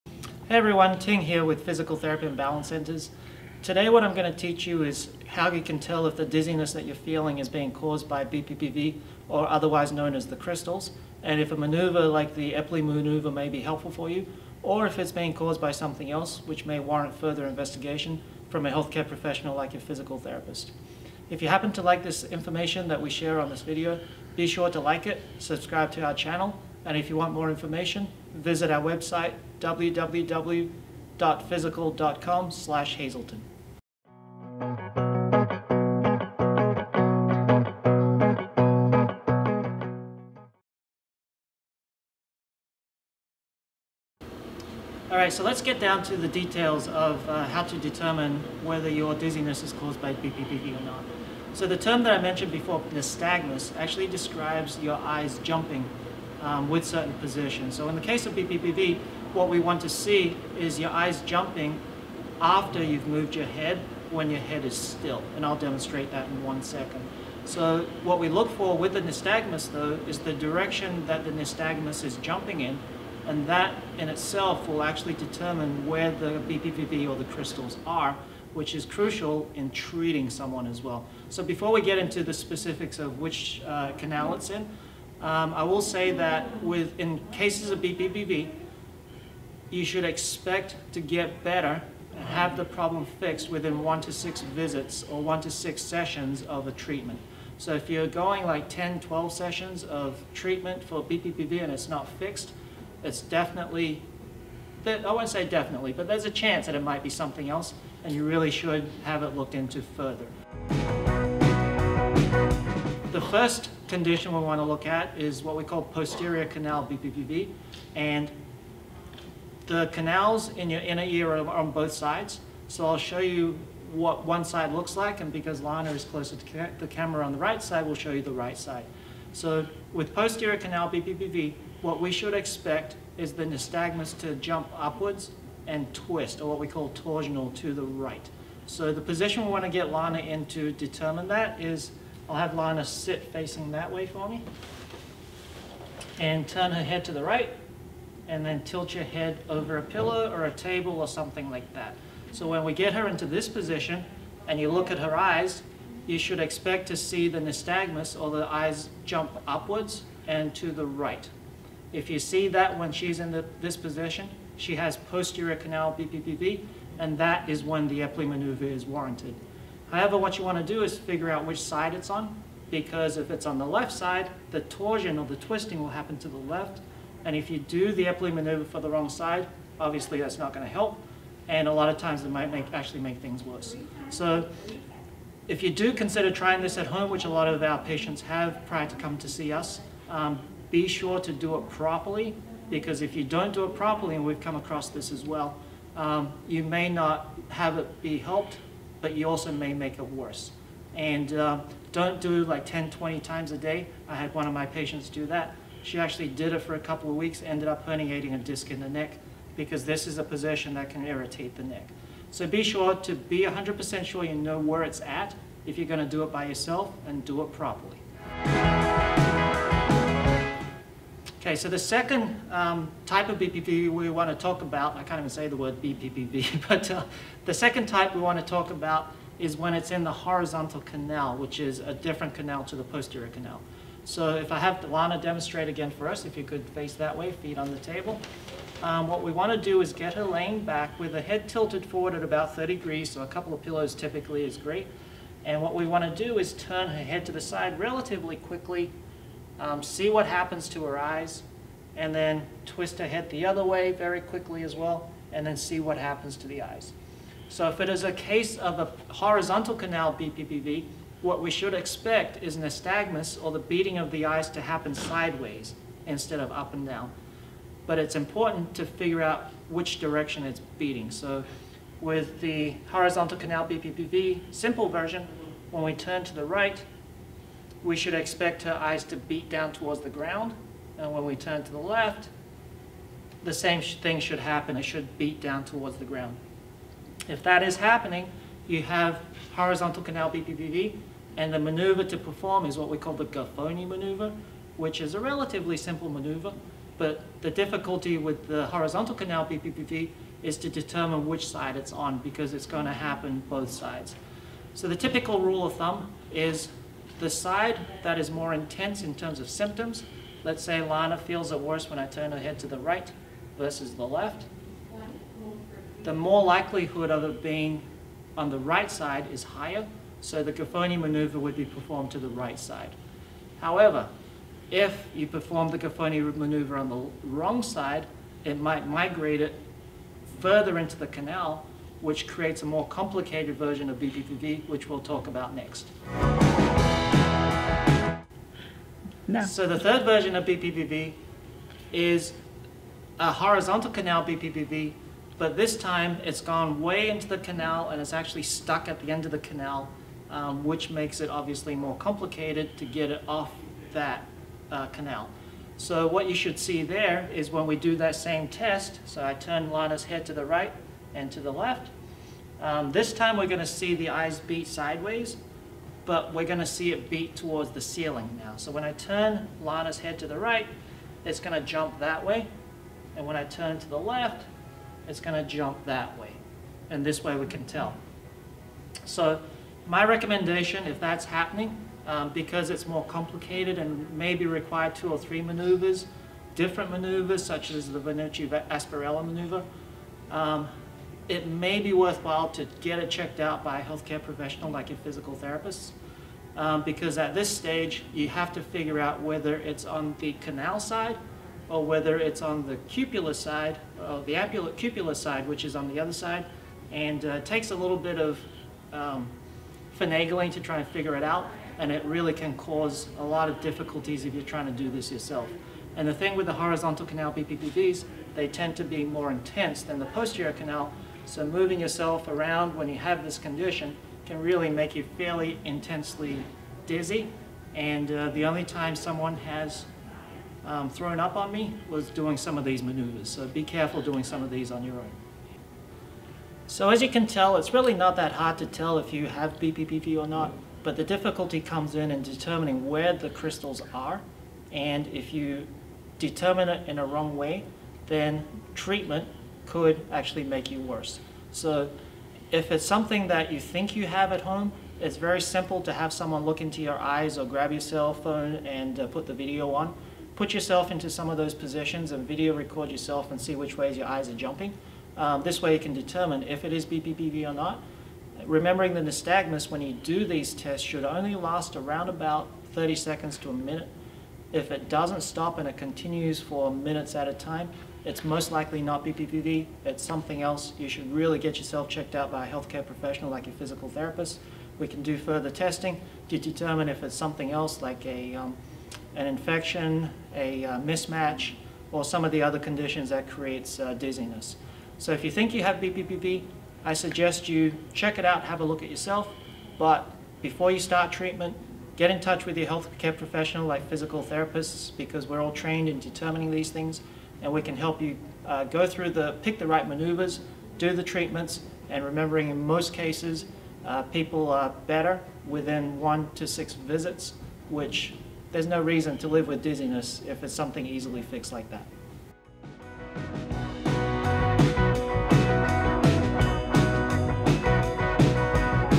How to tell if dizziness is caused by BPPV - when the Epley Should and shouldn't be done
shows you how you can tell if Dizziness is being caused by BPPV or otherwise known as the Crystals in the ear.

In this video you will learn:
- How to tell whether dizziness is being caused by BPPV
- If it is, how to tell where it is located
- When the Epley Maneuver should be done
- When the Gafoni Maneuver shoulder be done